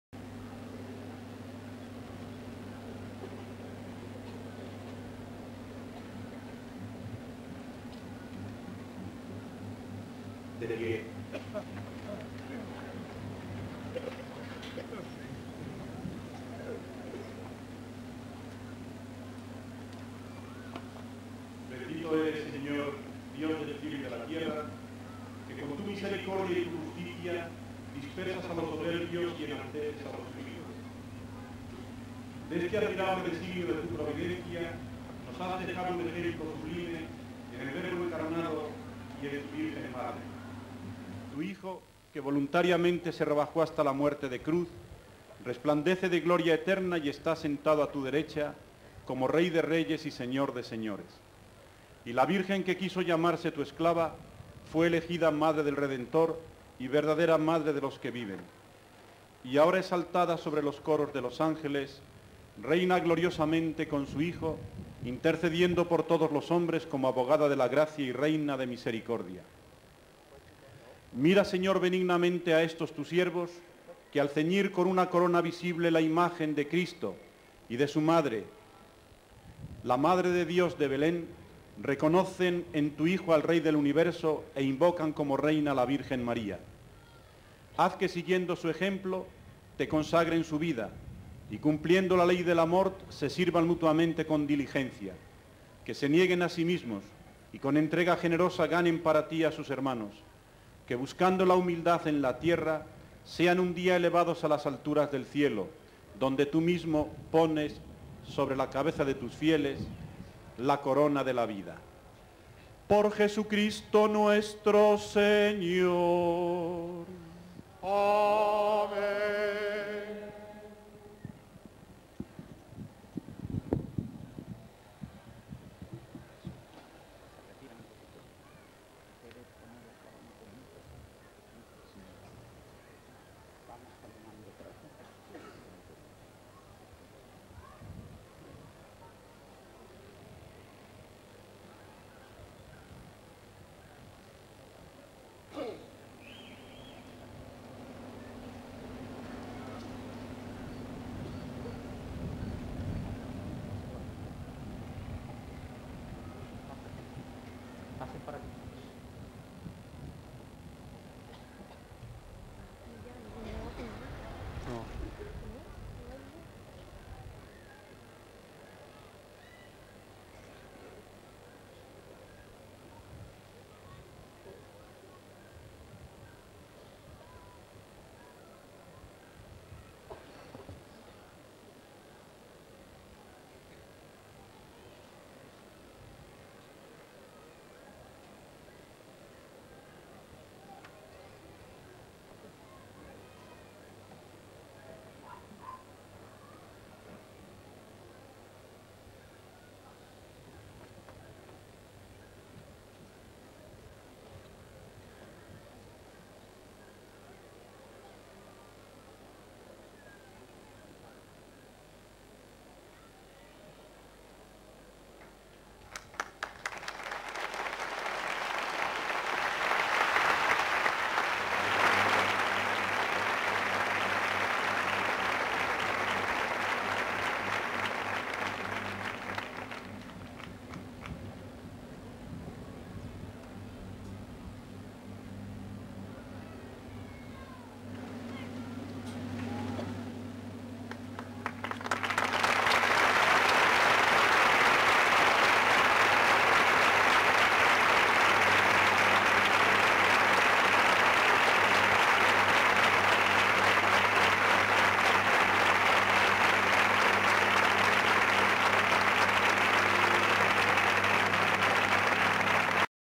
Coronación Canónica de la Virgen de Belén Pilas
Coronación Canónica de Ntra. Sra. de Belén de Pilas en el año 1996, coronada por Carlos Amigo Vallejo, Canal Pilas TV